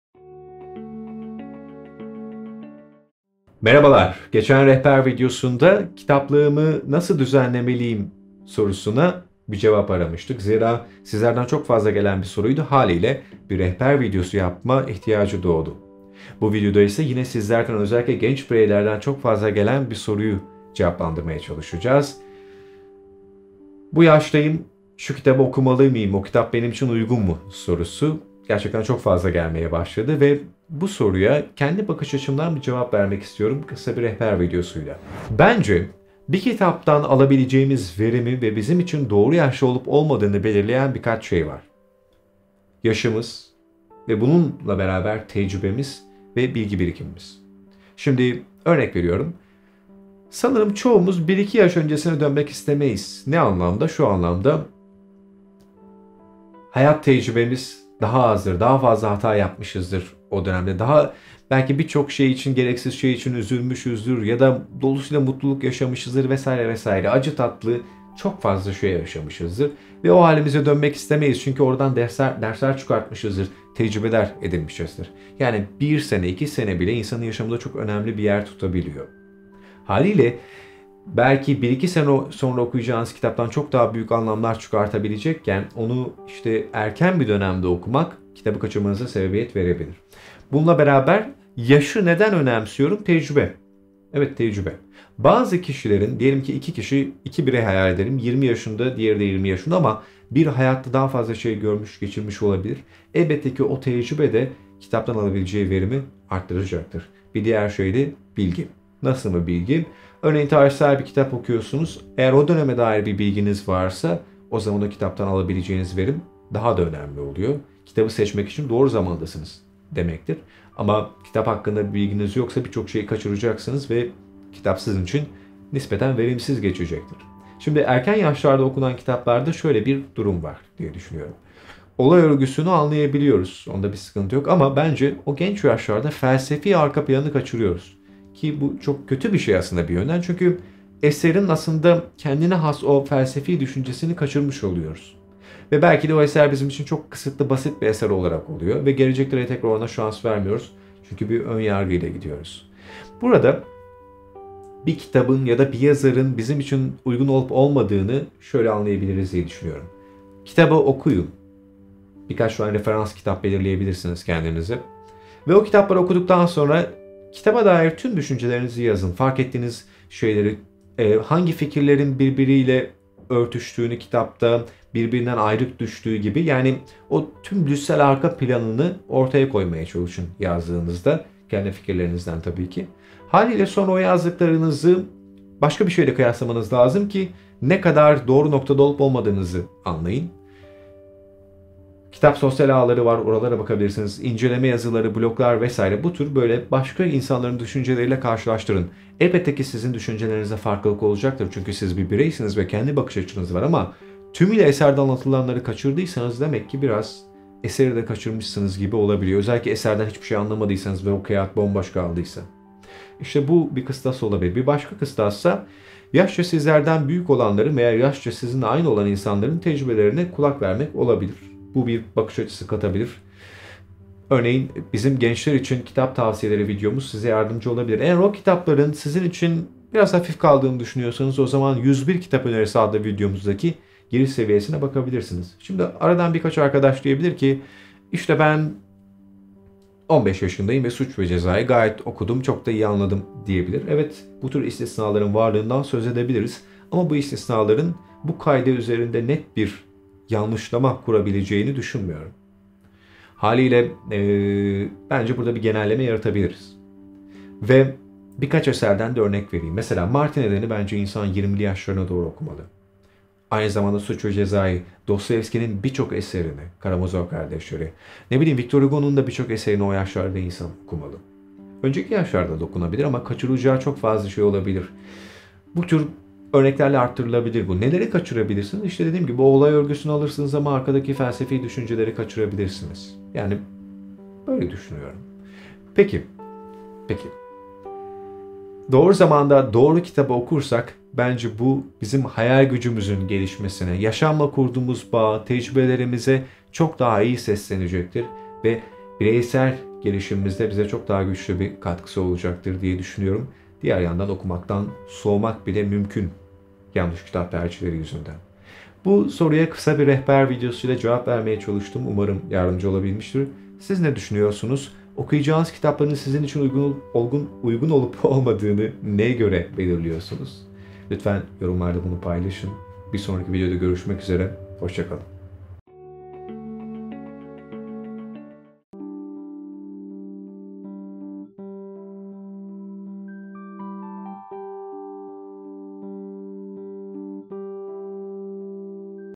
Hangi Kitap Hangi Yaşta Okunmalı?
"Şu yaştayım bu kitabı okumalı mıyım?" gibi soruları son dönemde çokça almaya başladık. Bu sorudan yola çıkarak kısa bir rehber videosuyla hangi kitap hangi yaşta okunmalı sorusuna cevap arıyoruz.